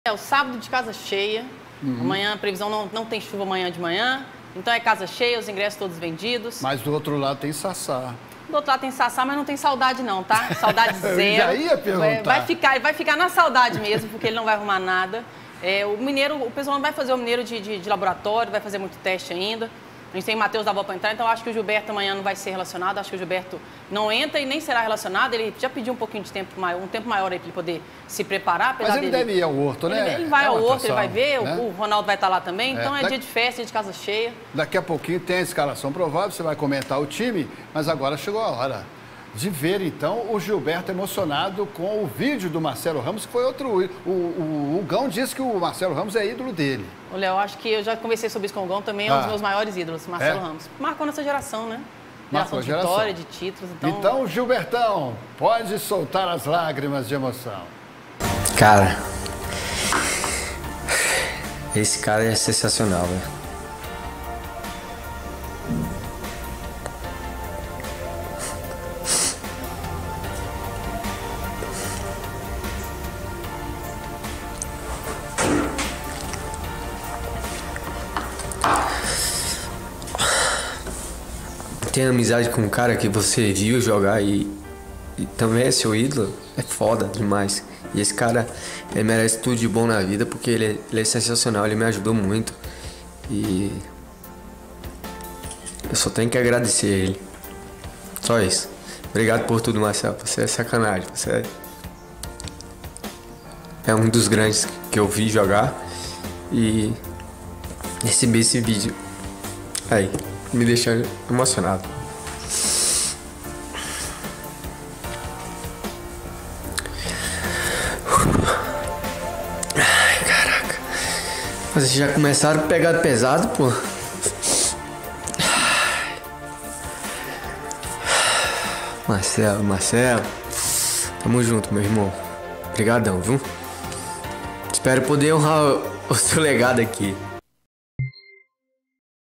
Gilberto estreia quando no Cruzeiro? Ana explica
O atacante Gilberto foi surpreendido com um vídeo do ídolo e ex-jogador do Cruzeiro Marcelo Ramos. O recado do ídolo celeste fez Gilberto se emocionar na Toca da Raposa.

"Esse cara é sensacional. Ter amizade com um cara que você viu jogar e também é seu ídolo... Esse cara merece tudo de bom na vida, ele é sensacional e me ajudou muito", disse.